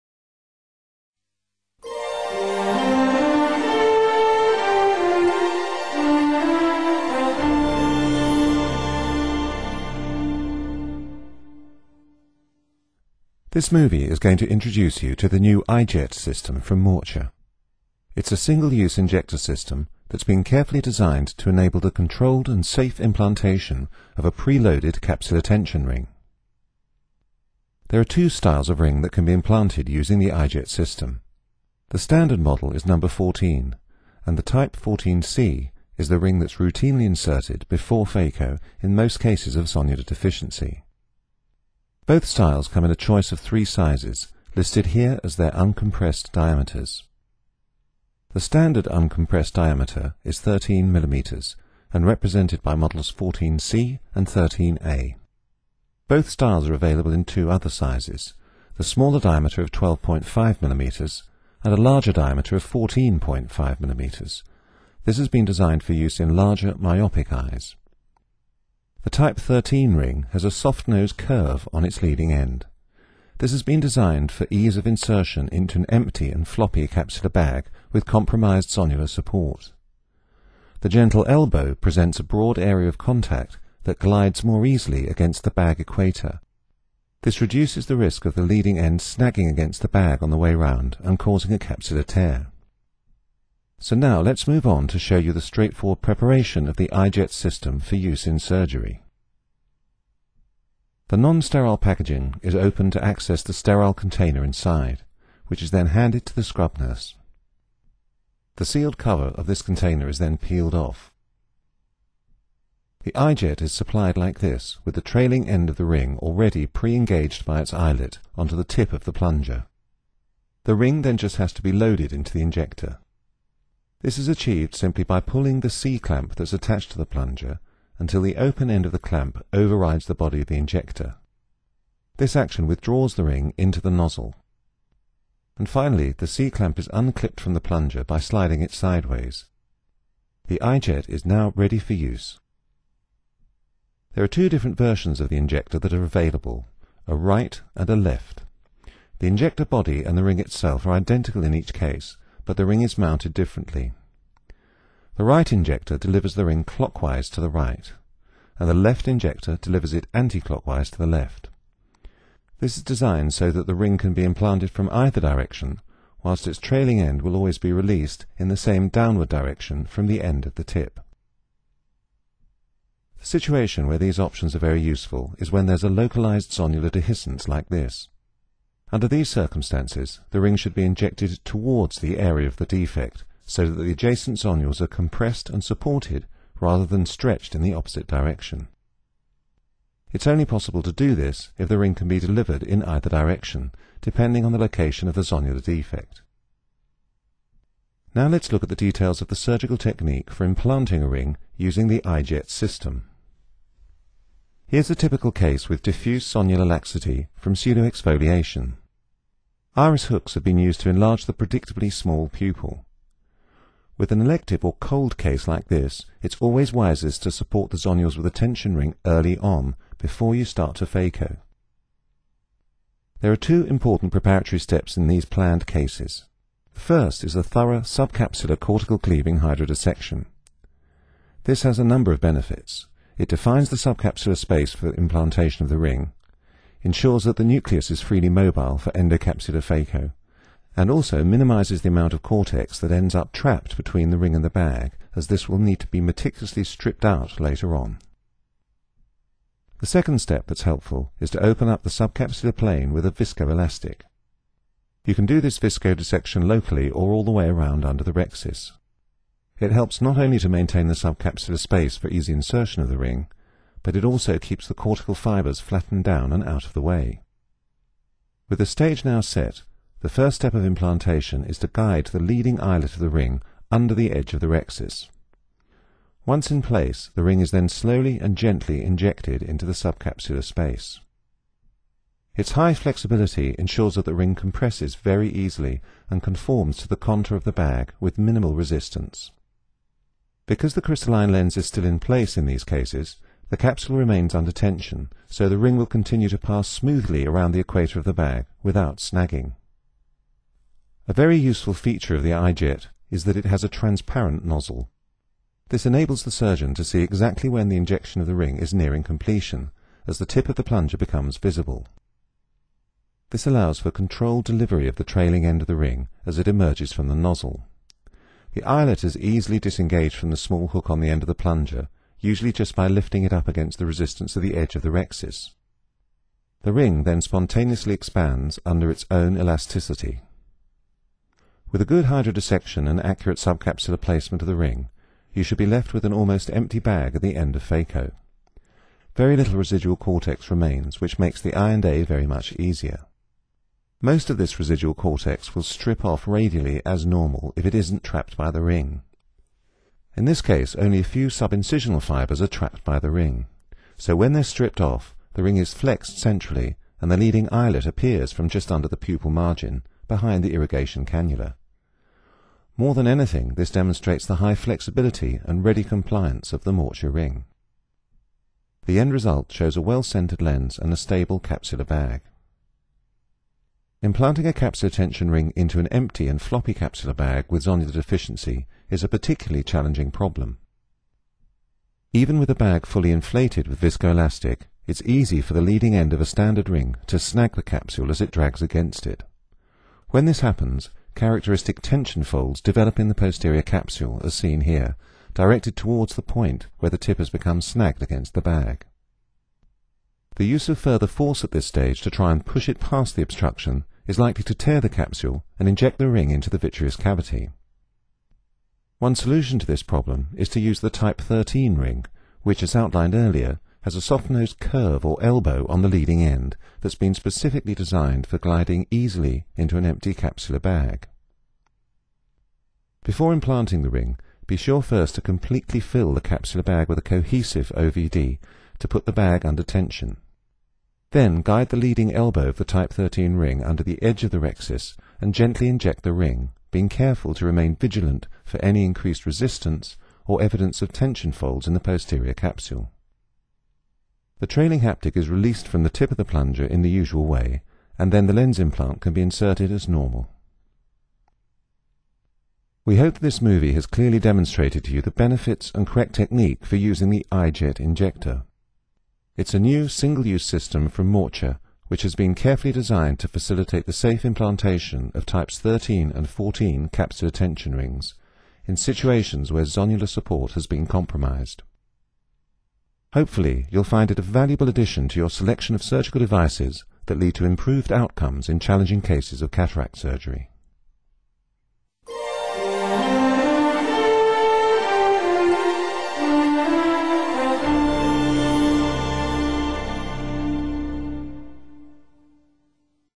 Morcher® EyeJet® | Capsular Tension Ring | Features & Procedure | FCI Cataract
Morcher® EyeJet® 14 and 15 Series| Preloaded Capsular Tension Ring (CTRs):

Designed by Morcher and distributed exclusively by FCI Ophthalmics, the Morcher EyeJet  CTRs allow you to convert complex cataract cases from compromised zonular integrity into routine cases.
Step-by-step instructions and size equivalence guide you in the choice and usage of the EyeJet® 14 and 15 Series.